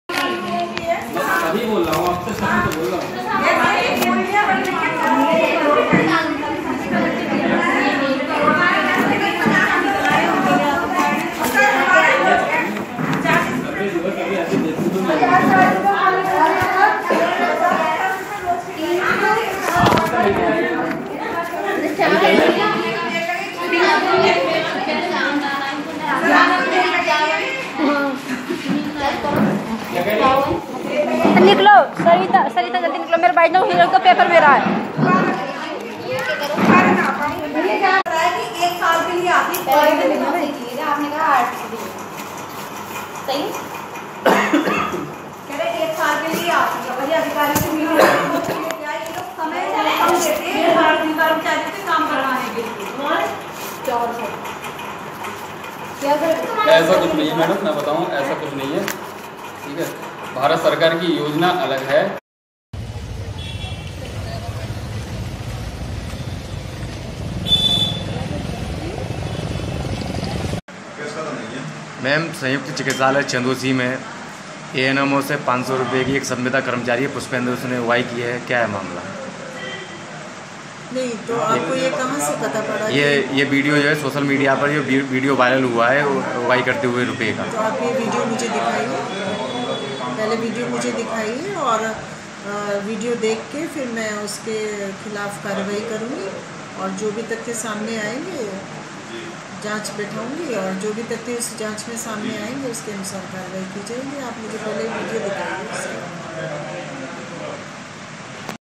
अस्पतालों में ₹500 की रिश्वत लेते इन रिश्वतखोर संविदा कर्मचारियों से कैसे निपटेंगे योगी जी. ?
यह रिश्वतखोर संविदा कर्मचारी आपसे परमानेंट होने की मांग करते हैं योगी जी ..@Sambhal| therawpost.com